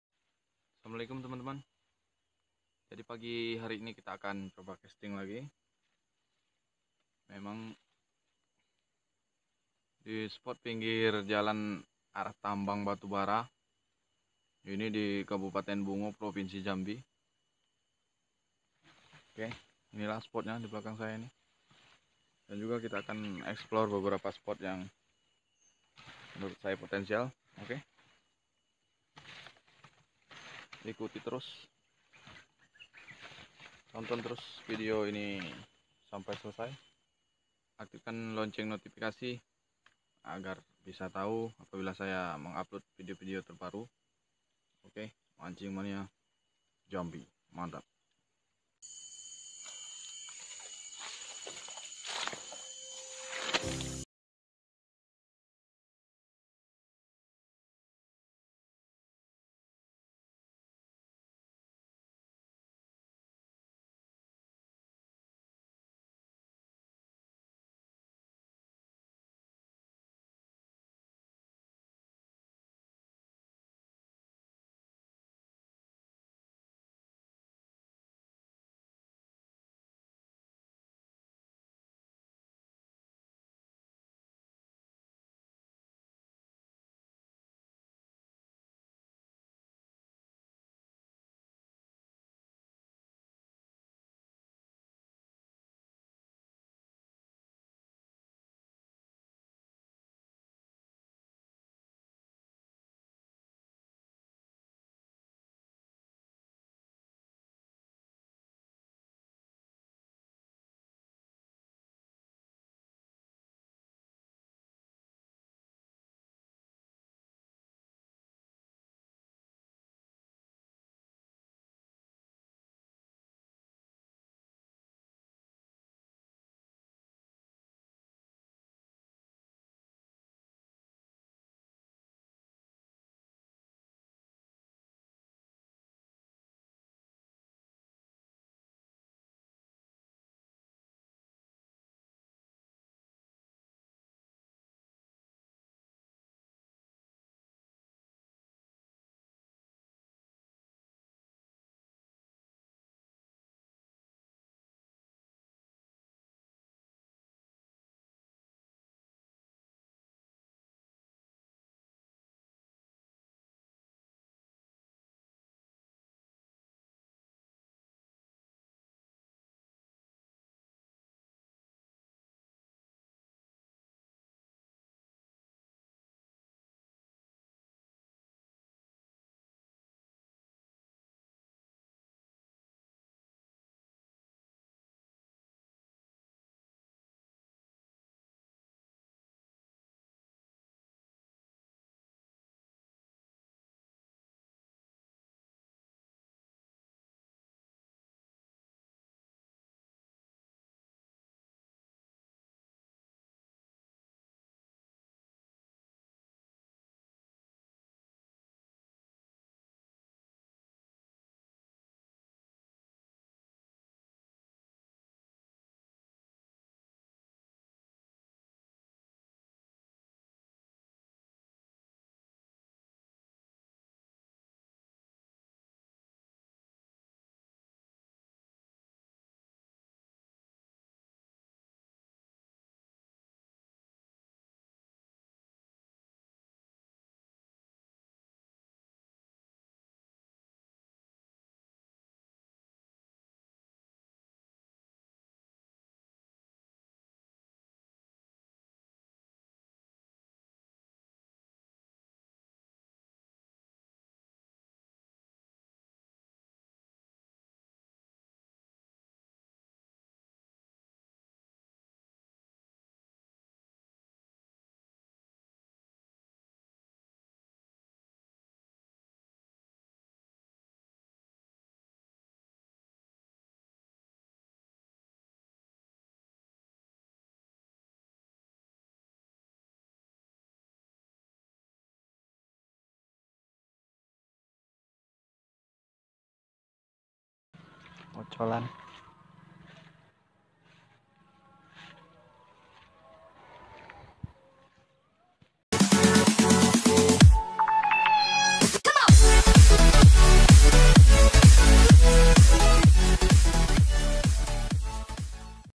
pagi pagi casting gabus pakai umpan soft frog || casting haruan 2020
casting gabus/haruan di pagi hari
hasilnya mantap

Lokasi : di tepi jalan batubara Muara bungo provinsi jambi
Lure : soft frog